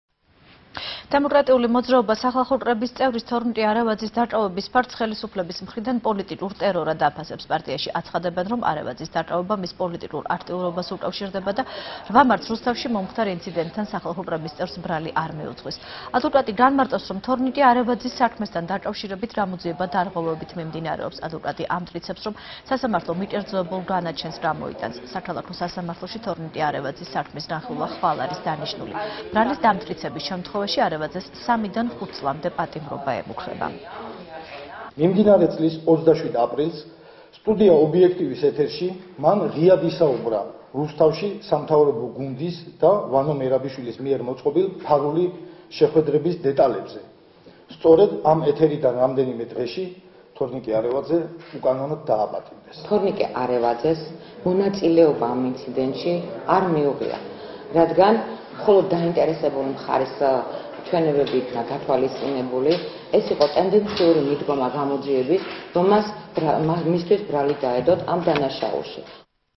ახალი 3 | დემოკრატიული მოძრაობა | 20.06.12
დემოკრატიული მოძრაობა სახალხო კრების წევრის, თორინკე არევაძის დაკავების ფაქტს ხელისუფლების მხრიდან პოლიტიკურ ტერორად აფასებს.  პარტიაში აცხადებენ, რომ არევაძის დაკავება მის პოლიტიკურ აქტიურობას უკავშირდება და 8 მარტს, რუსთავში მომხდარ ინციდენტთან  სახალხო კრების წევრს ბრალი არ მიუძღვის. ადვოკატი განმარტავს, რომ თორნიკე არევაძის საქმესთან დაკავშირებით გამოძიება დარღვევებით მიმდინარეობს. ადვოკატი ამტკიცებს, რომ სასამართლო მიკერძოებულ განაჩენს გამოიტანს. საქალაქო სასამართლოში თორნიკე არევაძის საქმის განხილვა ხვალ არის დანიშნული. ბრალის დამტკიცების შემთხვევაში არევაძეს 3-დან 5 წლამდე პატიმრობა ემუქრება.